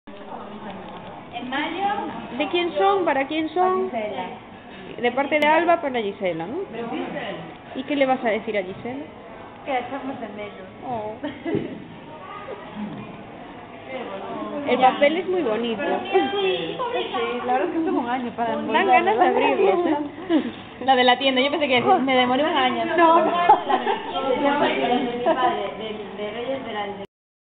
Cena de amigos invisibles R1 Familia - Mensaje de Alba
Mensaje de Alba dedicado a Gisella